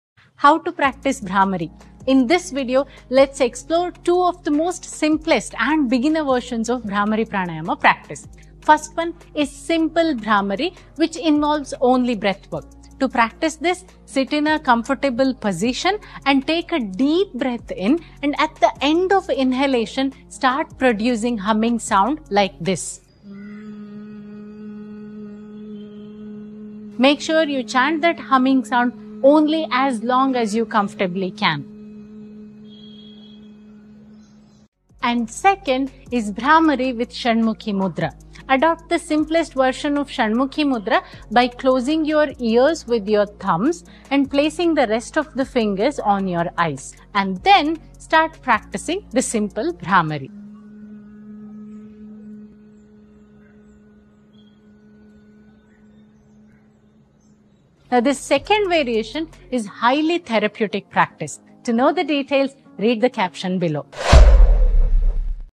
Right way to practice Brahmari Pranayama (Bumblebee Breath) ?
Last time, when we posted "Eye Health : Ep-3", we had people reaching out asking "how to practice Bhramari correctly?". So here's the video for the same. Watch it to learn about the beginner versions of BHRAMARI practice.

More info on the 2nd version:
Bhramari with Shanmukhi mudra is highly therapeutic. The practice of humming itself has a huge soothing effect on the nervous system. And the Shanmukhi mudra enhances the effect of chanting by manyfolds for 2 reasons:
1. The Mudra places a gentle pressure on the muscles in & around the eyes - that creates gentle vibration which enhances the relaxing effect.
2. Shanmukhi mudra automatically reduces the distractions which naturally develops a state of internal awareness. So it can be highly effective in reducing the stress as well as great preparation for meditation.

Because of these reasons, Bhramari with Shanmukhi mudra is highly recommended in case of stress-related/psychosomatic disorders, sleep disorders, anxiety etc.

NOTE: According to Hatha Yoga, BHRAMARI is one of the 8 KUMBHAKA. So original practice involves breath retention and Bandha. But in my opinion, that'd be an advanced practice for a total beginner to Yoga.

So here I am with 2 of the most basic and beginner-friendly variations of BHRAMARI. Try them and let me know how it went. Also write to us in the comments if you've any questions regarding the same.